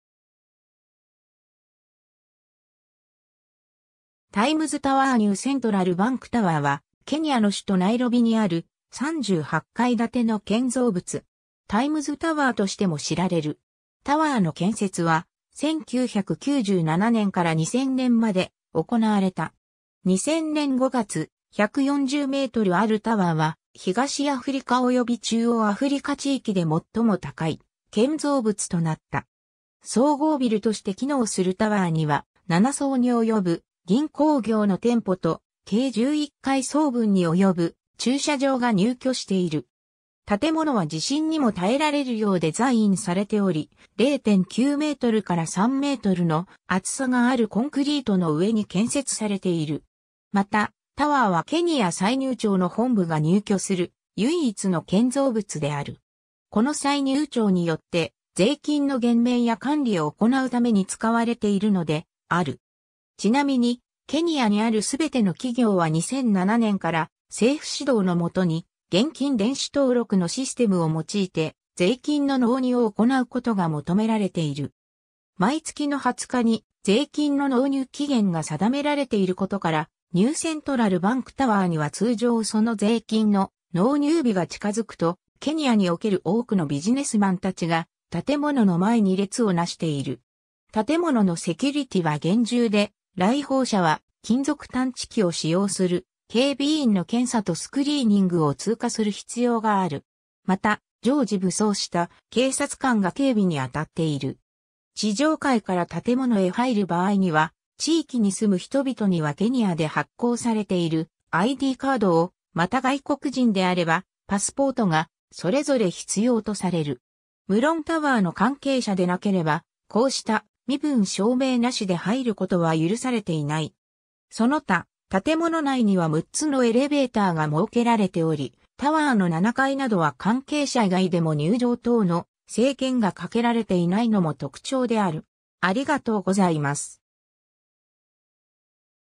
ニュー・セントラル・バンク・タワー
タイムズ・タワー ニュー・セントラル・バンク・タワー（英文表記: New Central Bank Tower）は、ケニアの首都ナイロビにある、38階建ての建造物。
タイムズ・タワー (Times Tower) としても知られる。
タワーの建設は、1997年から2000年まで行われた。
2000年5月、140メートルあるタワーは、東アフリカおよび中央アフリカ地域で最も高い建造物となった。
総合ビルとして機能するタワーには、7層に及ぶ銀行業の店舗と、計11階層分に及ぶ駐車場が入居している。
建物は地震にも耐えられるようデザインされており、0.9メートルから3メートルの厚さがあるコンクリートの上に建設されている。
 また、タワーはケニア歳入庁 (Kenya Revenue Authority, 略称KRA) の本部が入居する、唯一の建造物である。
この歳入庁によって、税金の減免や管理を行うために使われているのである。
ちなみに、ケニアにある全ての企業は2007年から、政府指導のもとに現金電子登録のシステムを用いて税金の納入を行うことが求められている。
 毎月の20日に税金の納入期限が定められていることから、ニュー・セントラル・バンク・タワーには通常その税金の納入日が近づくと、ケニアにおける多くのビジネスマン達が建物の前に列をなしている。
 建物のセキュリティは厳重で、来訪者は金属探知機を使用する警備員の検査とスクリーニングを通過する必要がある。
また、常時武装した警察官が警備に当たっている。
地上階から建物へ入る場合には、地域に住む人々にはケニアで発行されているIDカードを、また外国人であればパスポートが、それぞれ必要とされる。
無論タワーの関係者でなければ、こうした身分証明無しで入ることは許されていない。
 その他、建物内には6つのエレベーターが設けられており、タワーの7階などは関係者以外でも入場等の制限が掛けられていないのも特徴である。